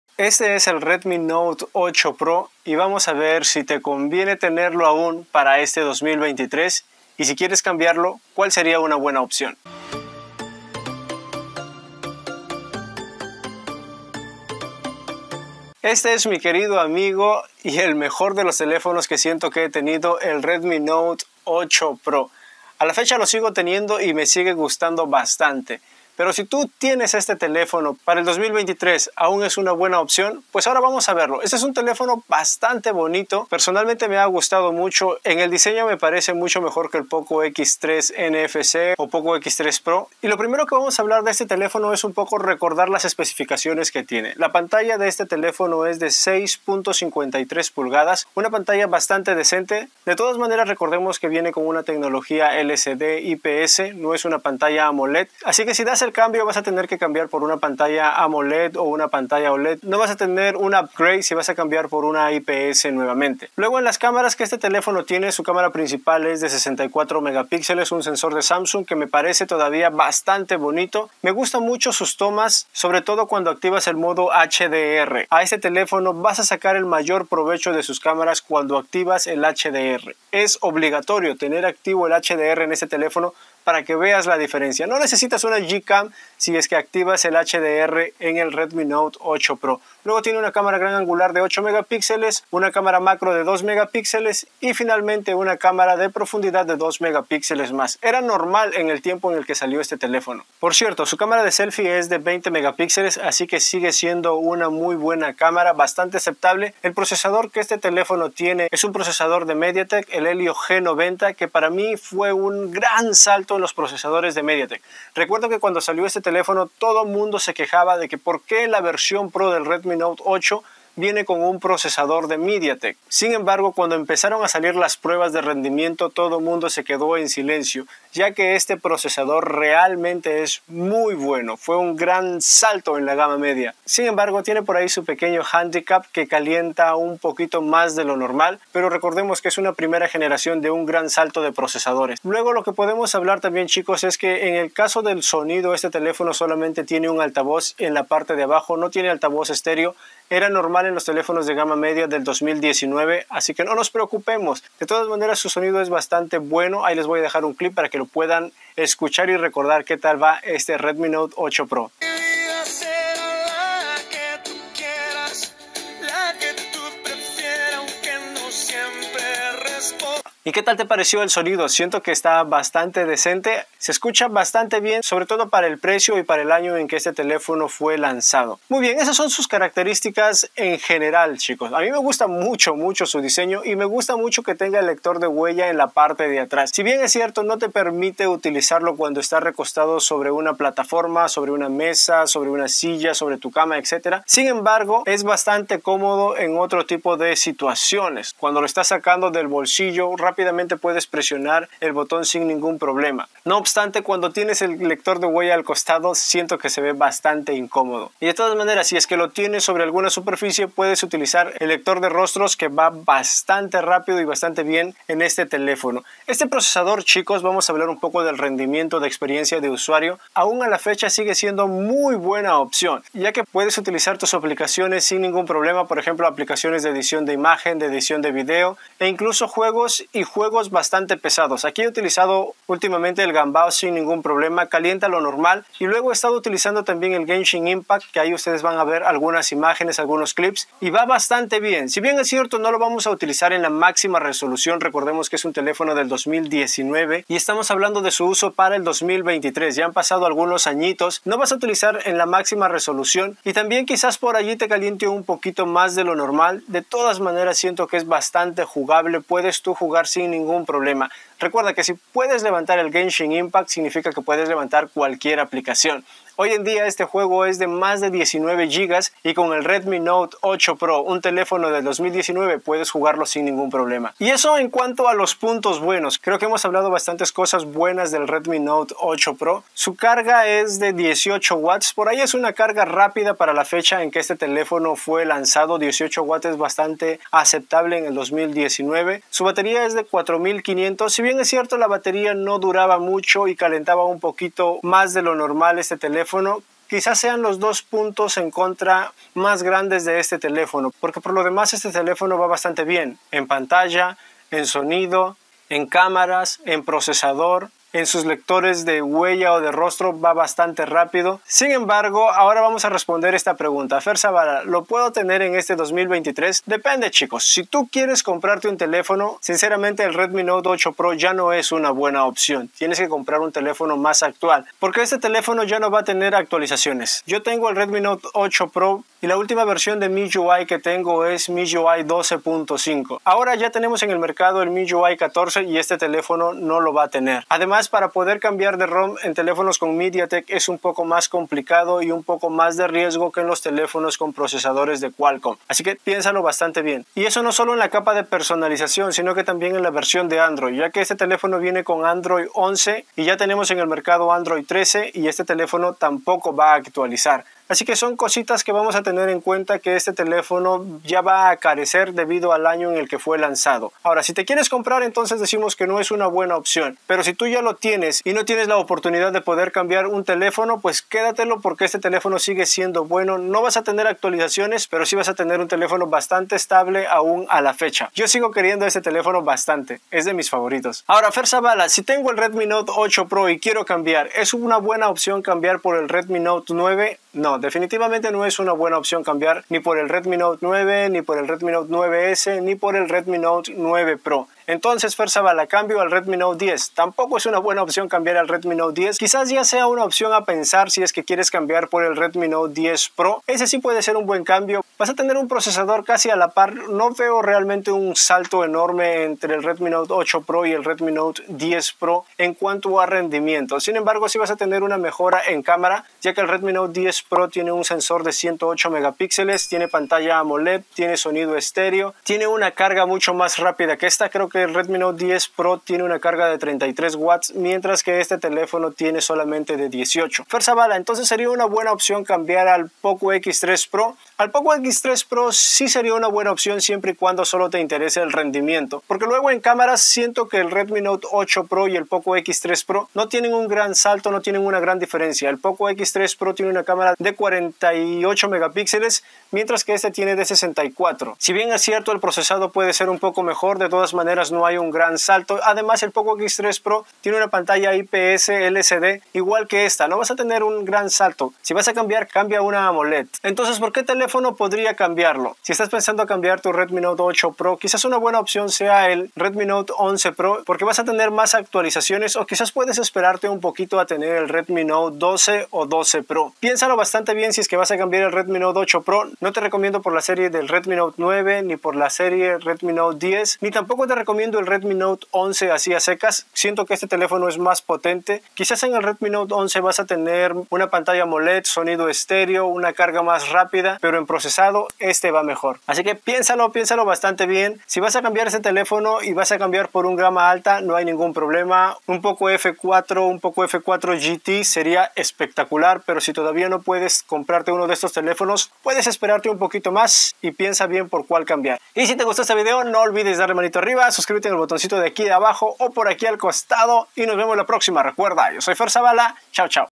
Redmi Note 8 Pro - ¿Vale la Pena para el 2023? - Tienes que ver este video
Tenemos en el Canal al Redmi Note 8 Pro y queremos que tan bien nos puede ir para este 2023, acompáñame a ver este video